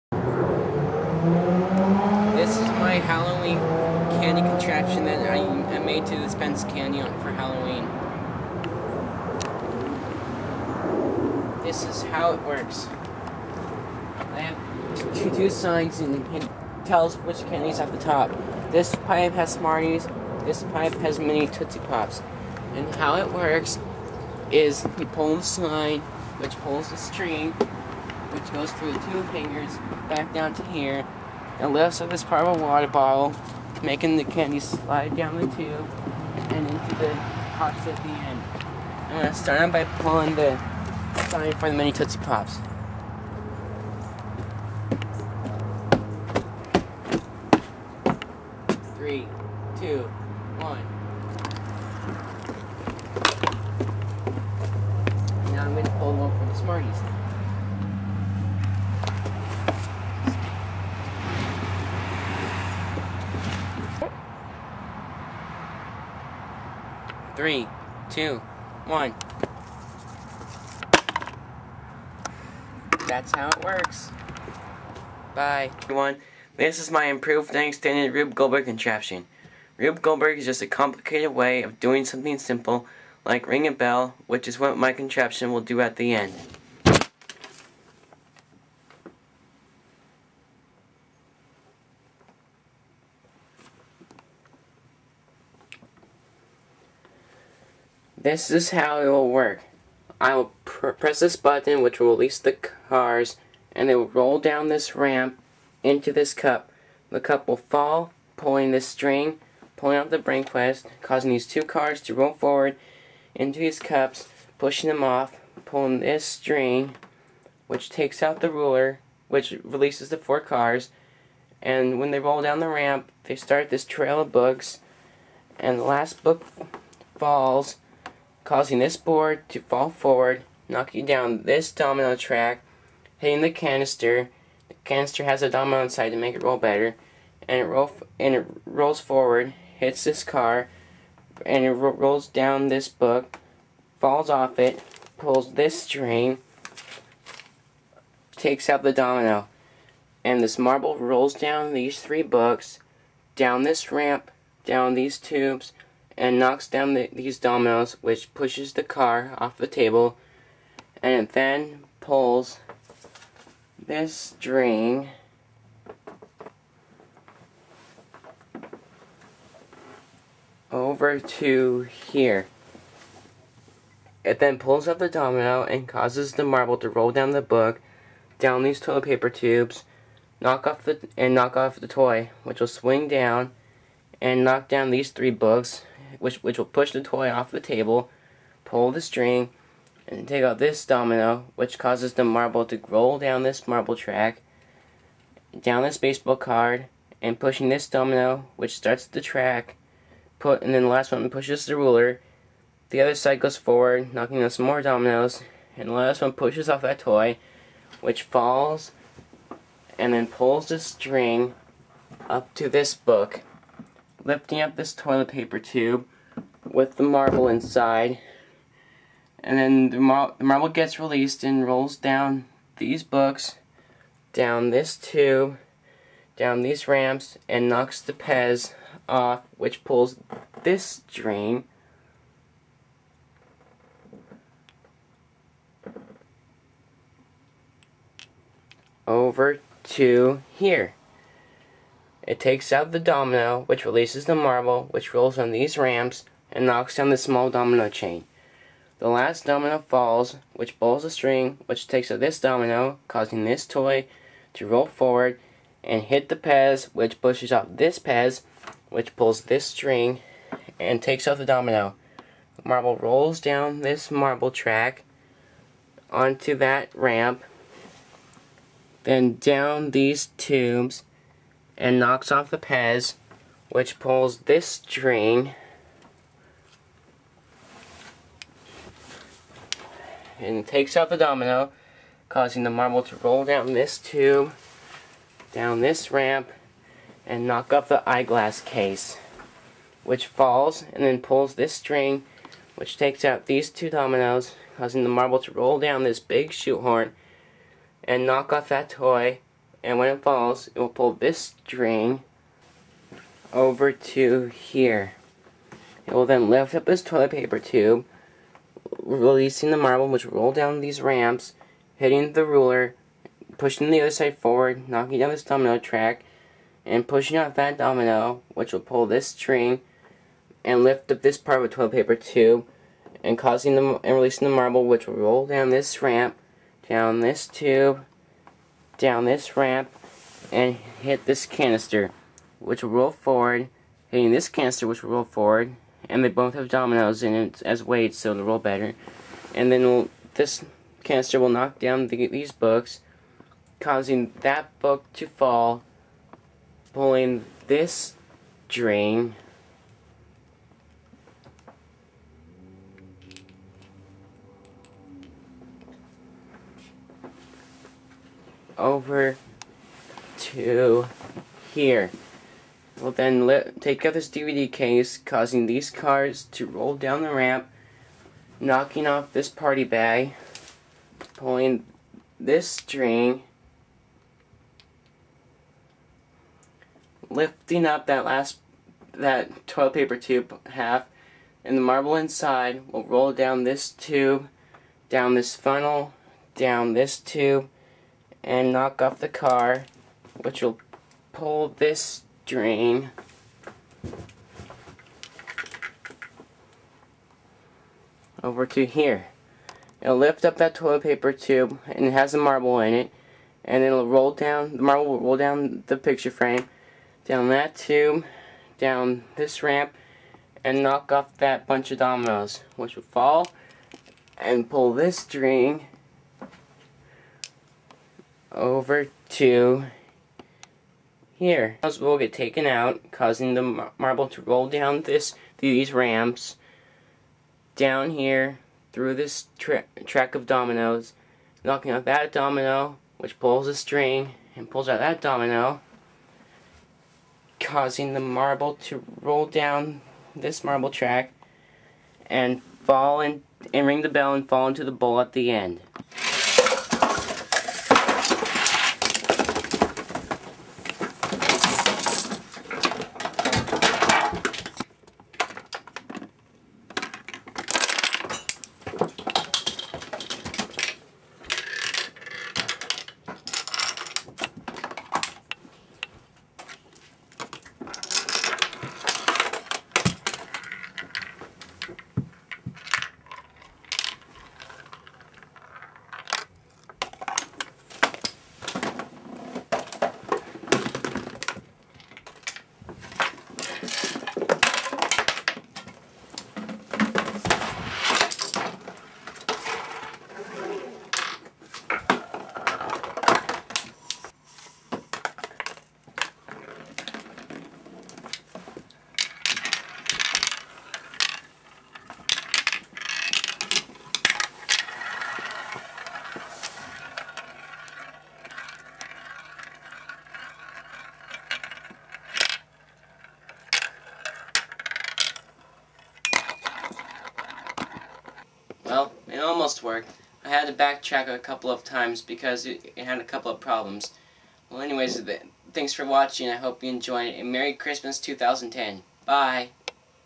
My Halloween Contraption & Rube Goldberg (w/ captions)
This is my first YouTube video. The first video on this is my Halloween Candy Contraption which was done in November 2010. The next video is my Rube Goldberg Contraption in my bedroom. I recorded it on Christmas Eve 2010. (Hope you like it)